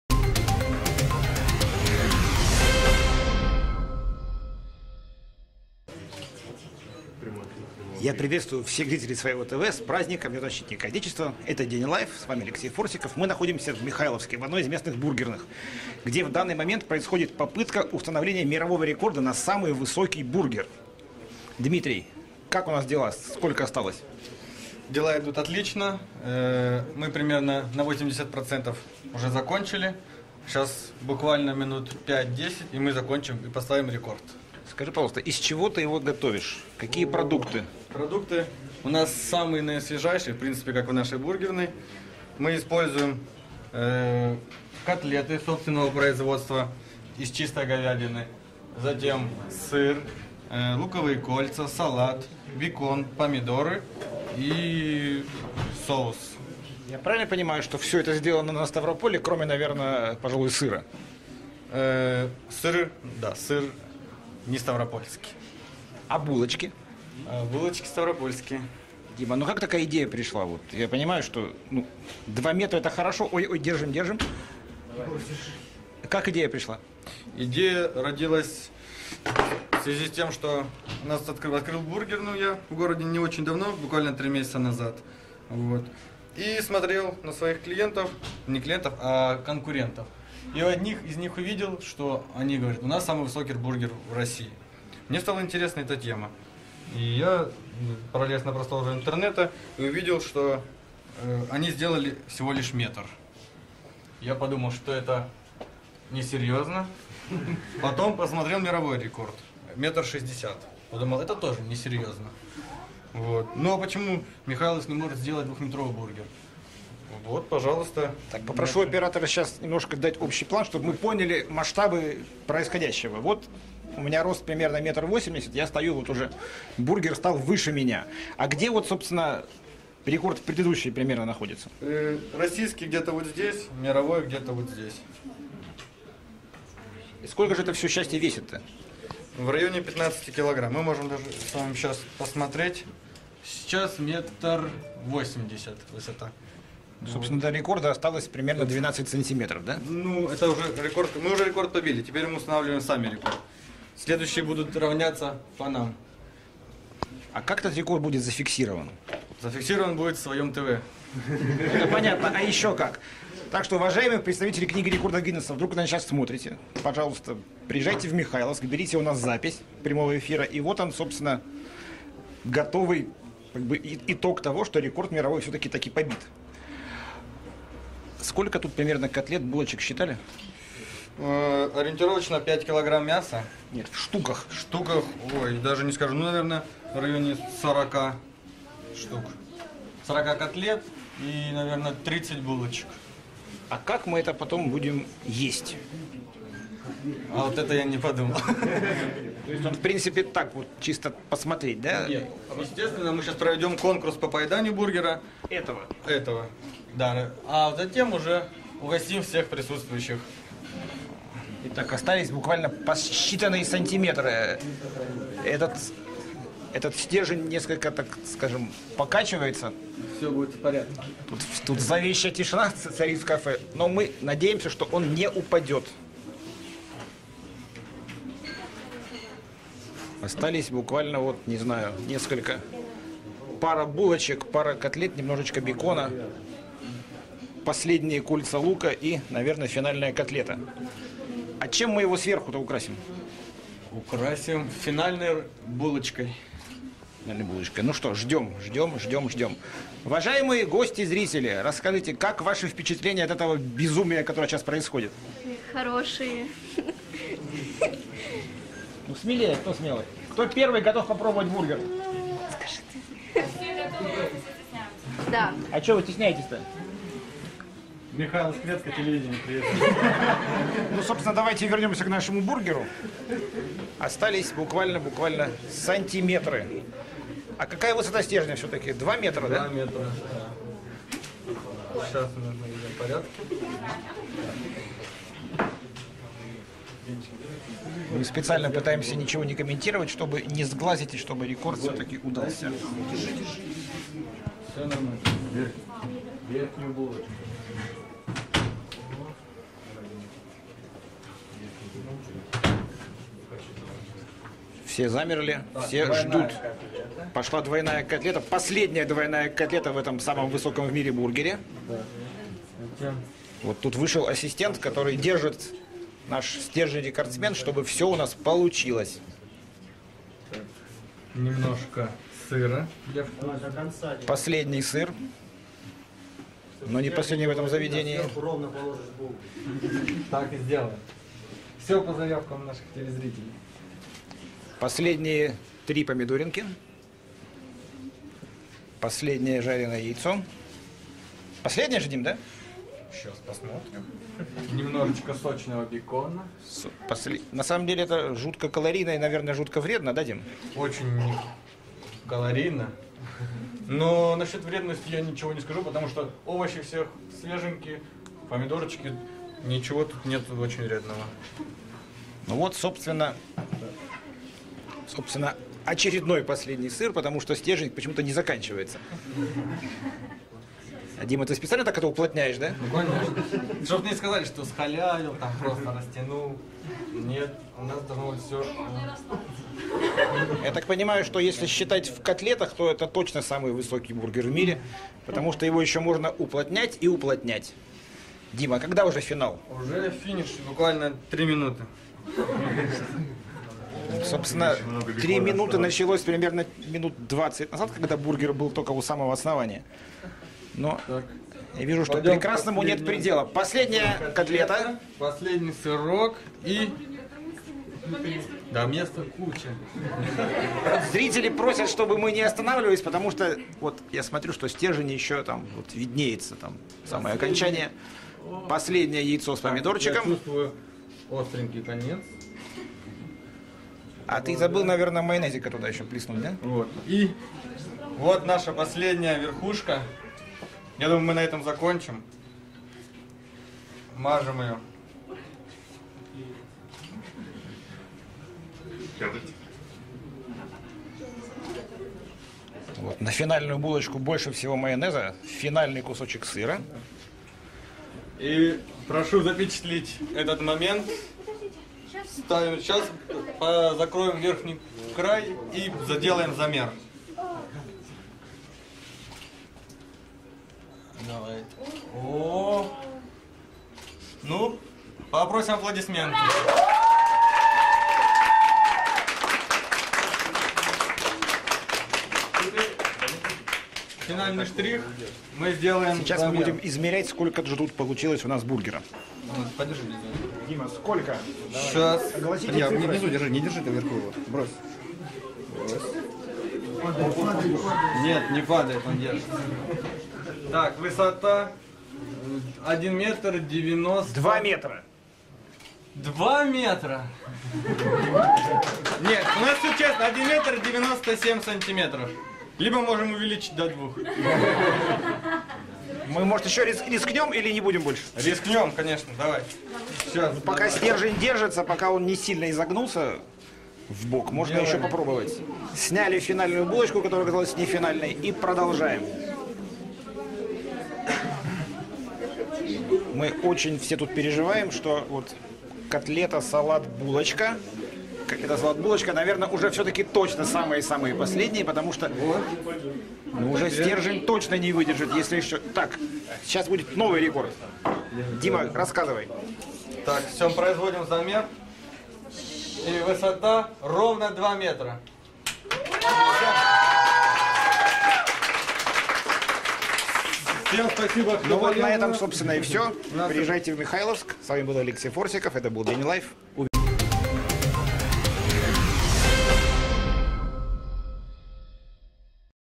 День Live: В Михайловске приготовили двухметровый бургер
Бургер высотой почти 2 метра приготовили в одном из кафе Михайловска. Пять поваров соорудили блюдо, каких ранее не делали не только в Ставропольском крае, но и во всём мире.